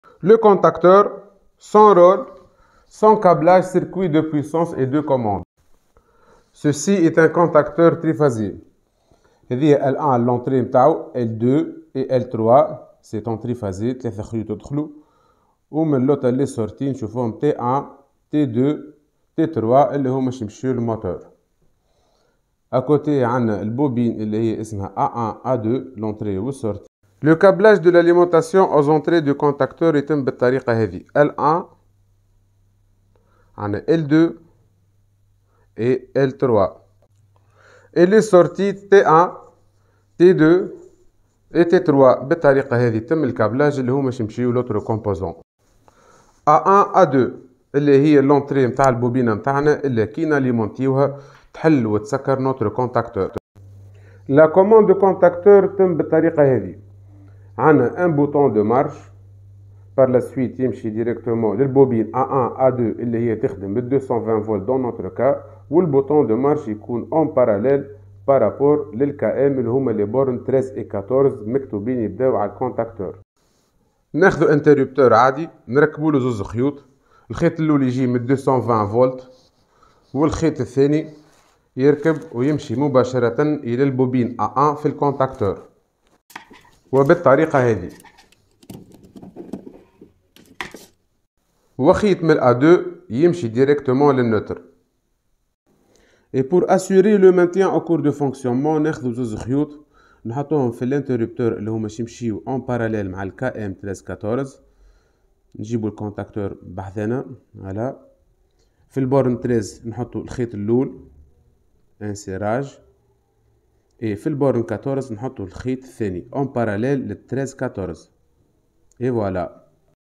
le contacteur, circuit de commande et circuit de puissance
connaître de quoi il s'agit le contacteur dans un circuit électrique, et comment l'installer